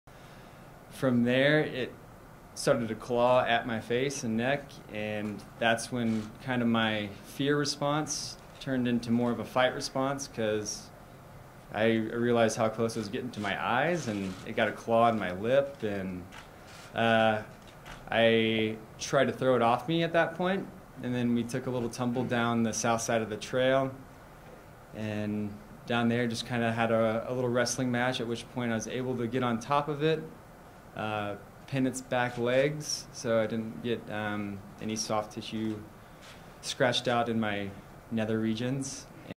Man who killed mountain lion with bare hands explains how he won ‘wrestling match’
Could a music-free run have been the difference between life and death for a Colorado man who made headlines last week by killing a mountain lion with his bare hands?

Travis Kauffman lived to tell about his encounter with a mountain lion at Horsetooth Mountain Open Space in Larimer County.

Kauffman said on Thursday he was not wearing headphones during his run on Feb. 4 - which allowed him to hear the big cat's approach.

He described his reaction when the mountain lion attacked and how he fought it off.

“It started to claw at my face and neck and that is when kind of my fear response turned into more of a fight response,” he said. “Because I realized how close it was getting to my eyes and it got a claw in my lip.”

He said he and the mountain lion basically began to wrestle down the trail.

“I tried to throw it off of me at that point, and then we took a little tumble down the southside of the trail and down there just kind of had a little wrestling match,” he said. “At which point I was able to get on top of it, pin its back legs so I didn't get any soft tissue scratched out in my nether regions."

Kauffman ran three miles through the trail after killing the animal.

Fortunately, he encountered other joggers and one of them gave him a ride to the hospital - where he received 20 stitches for cuts.

Two other mountain lions were found in the same area and removed just a few days after the attack.

Kauffman, 31, admitted, “There was a point where I was concerned I wasn’t going to make it out.”

He did, and now he’s famous. But he’s trying to downplay his new mountain man, survivalist image.

“I will never be able to live up to the reputation. And maybe that’s what has led to some of my reticence for actually coming out," he said. "The story is bigger than my puny form.”